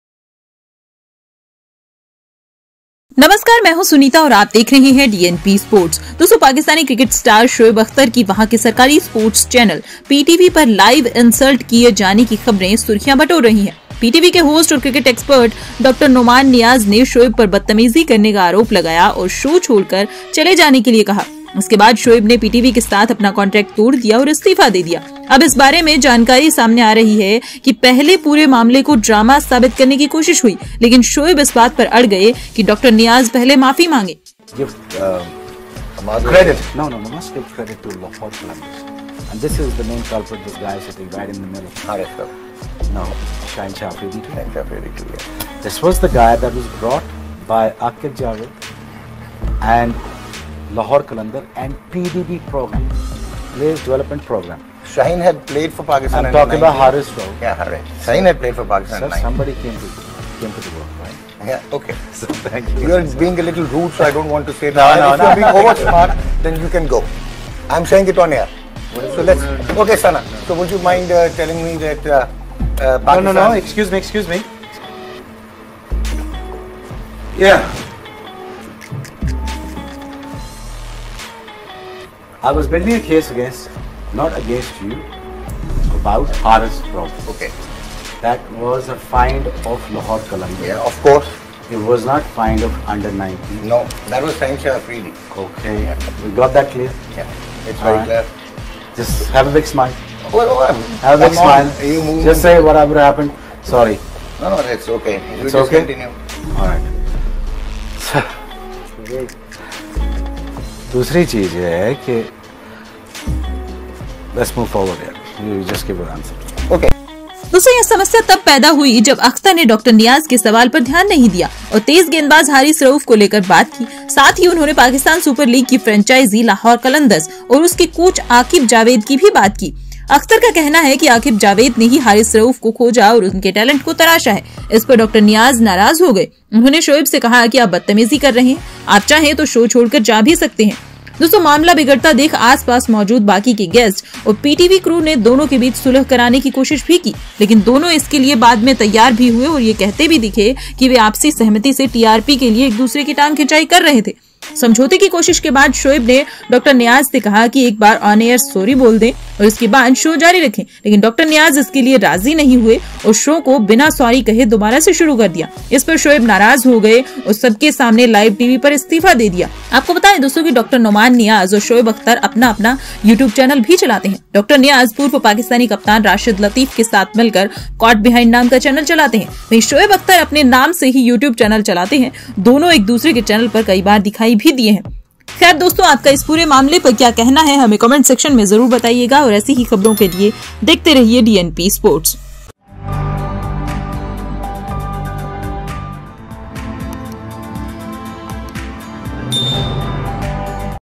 पाकिस्तान के नेशनल स्टार की लाइव शो के दौरान की गई बेइज्ज़ती,क्या ये केवल ड्रामा था? || DNP Sports
शोएब अख़्तर की बेइज्ज़ती
ड्रामा
  या
सच्चाई?